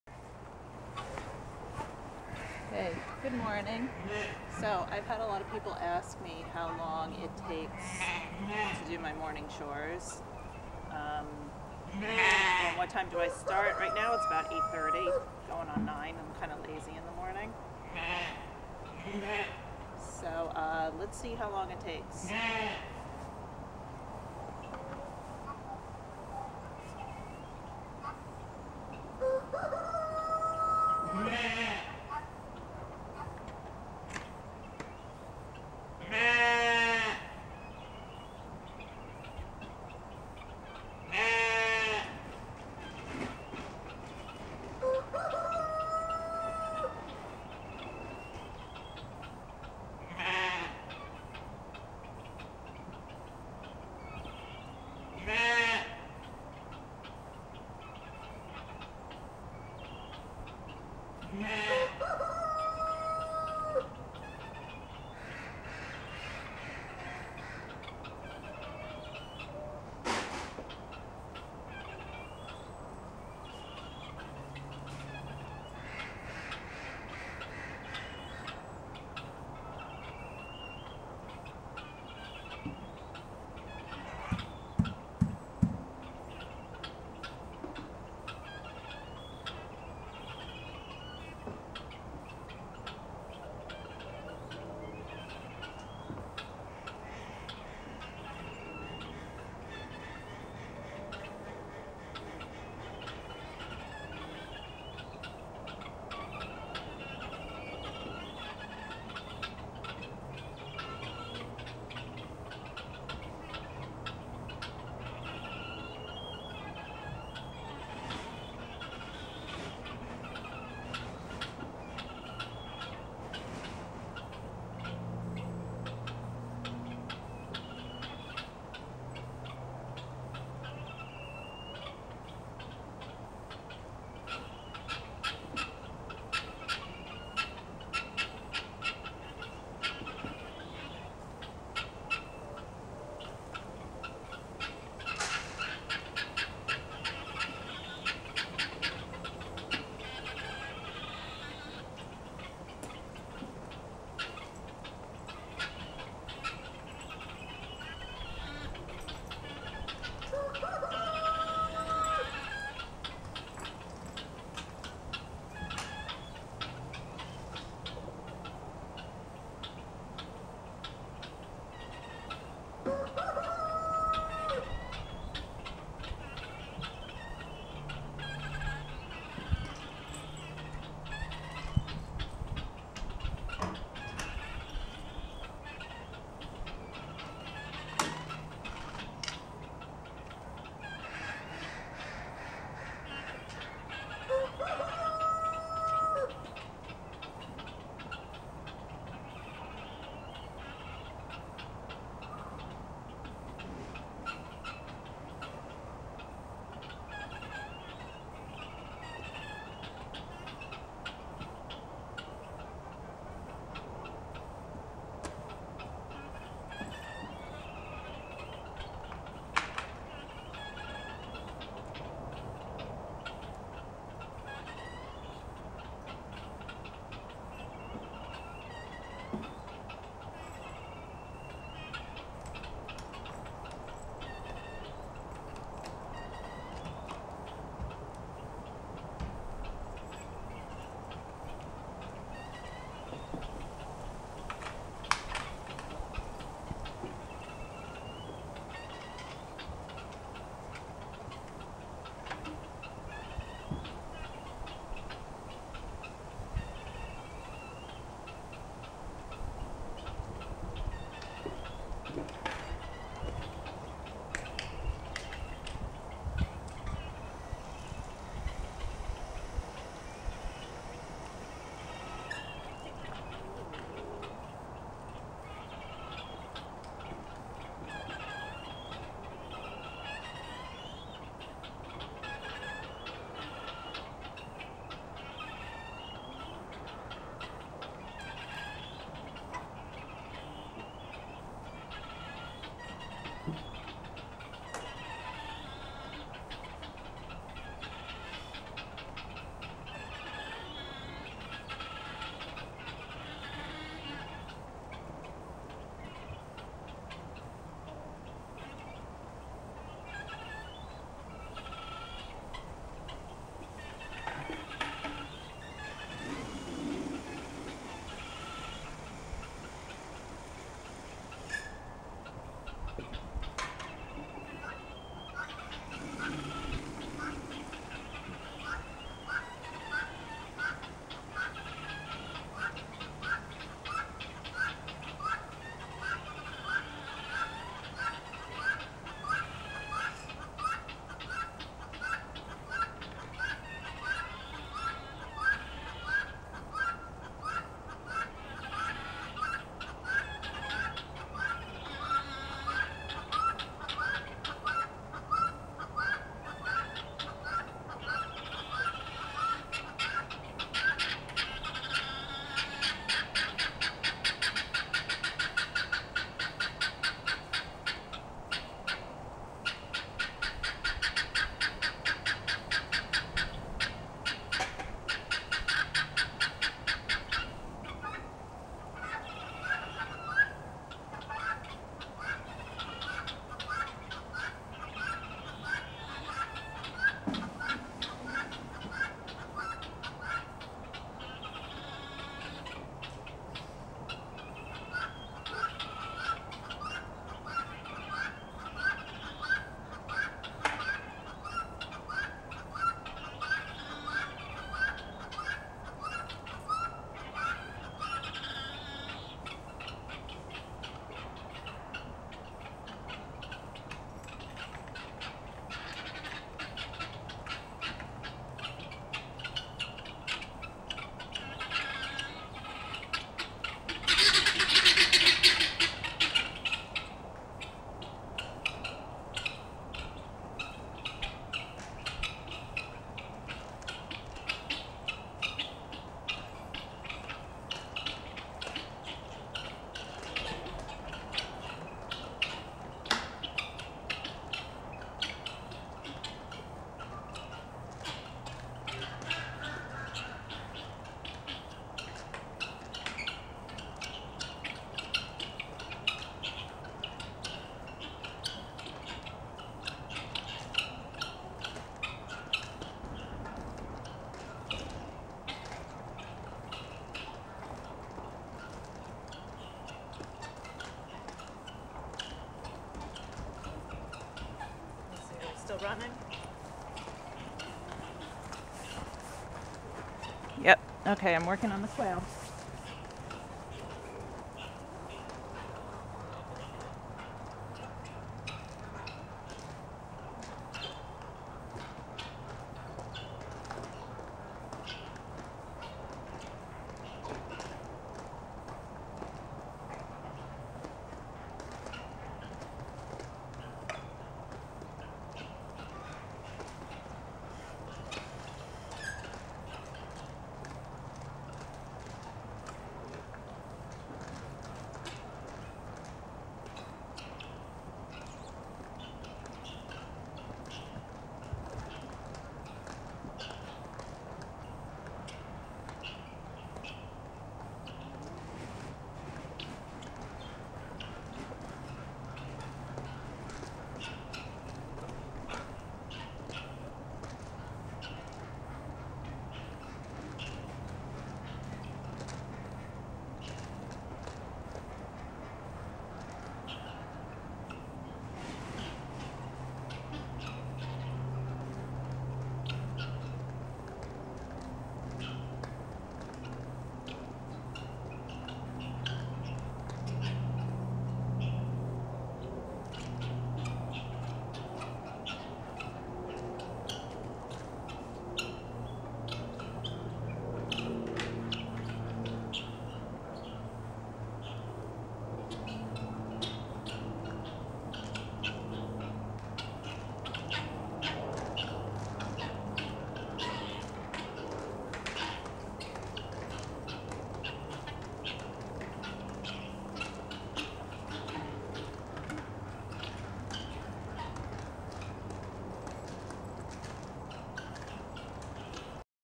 Morning Chores Part 1
Video of me doing my morning chores.  Notice the PJs.  NOT A MORNING PERSON!